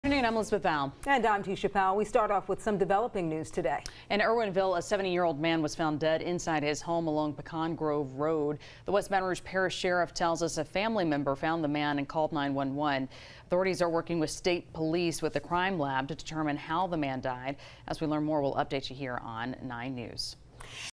Elderly man found dead in WBR home
A 70-year-old man was found dead inside his home along Pecan Grove Road in Erwinville Tuesday, July 11.